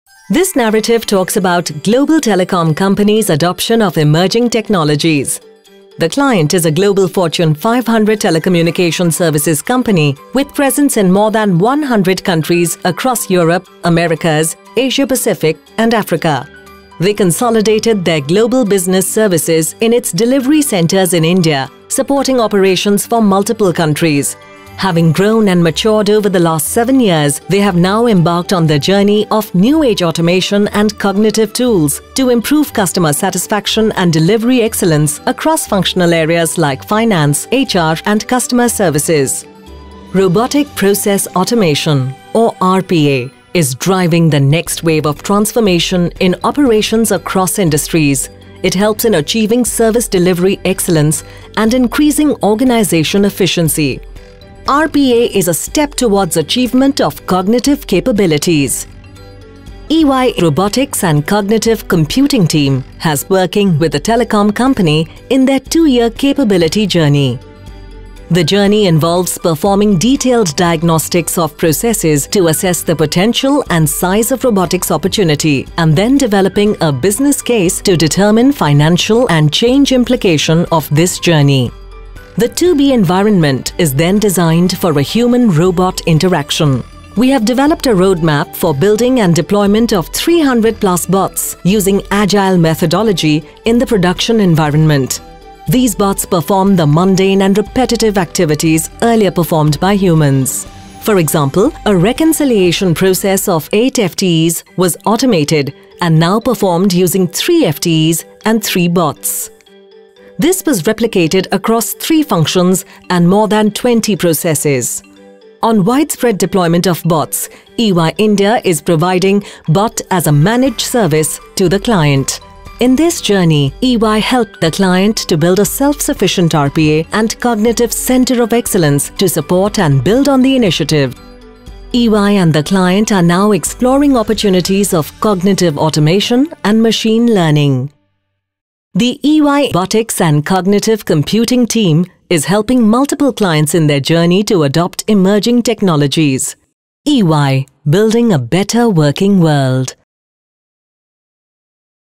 Connecting the Globe: The Robotics Opportunity in Telecommunication
Today’s businesses, government and individuals are responding to shifts that would have seemed unimaginable even a few years ago. With the next wave of disruption ahead of us, the future promises to be even more revolutionary with the rapid emergence of Robotic Process Automation or RPA which is transforming the way businesses function.  Watch this video to know how EY’s Robotics and Cognitive Automation team helped a telecom major accelerate its digital journey by deploying 300+ BOTs and designing an advanced environment to enable a human – robot interaction. EY is increasingly working with clients to explore Cognitive automation opportunities and proof-of-concept in areas such as image matching and machine learning.